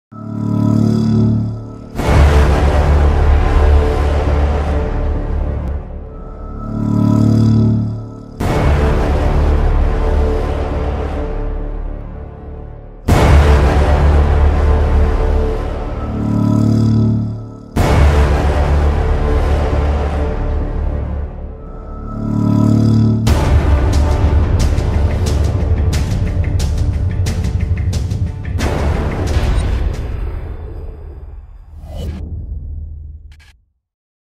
Cell Trailer (CairoScene Parody)
Our resident king of the nerds, Skot Thayer, discovers his cellphone can kill the people who annoy him at the office, much like John Cusack killed his career in his new movie Cell, just out in Egypt.

Check out the trailer for the actual film and find out where and when to watch it on our Cinema Guide. Follow us on